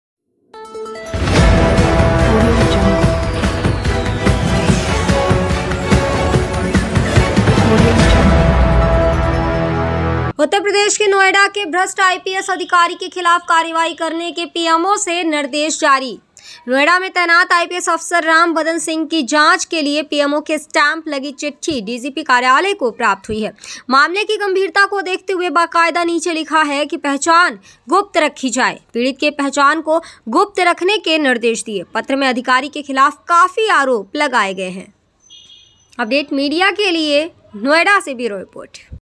उत्तरप्रदेश के नोएडा के भ्रष्ट IPS अधिकारी के खिलाफ कार्रवाई करने के PMO से निर्देश जारी...
नोएडा में तैनात IPS अफ़सर  राम बदन सिंह की जाँच के लिए पीएमओ की स्टैम्प लगी चिट्ठी DGP कार्यालय को प्राप्त हुई है।मामले की गम्भीरता को देखते हुए बाक़ायदा नीचे लिखा है कि पहचान गुप्त रखी जाए।

पीड़ित के पहचान को गुप्त रखने के निर्देश...पत्र मे अधिकारी के खिलाफ काफी आरोप लगाए गये हैं.

So , Thankyou !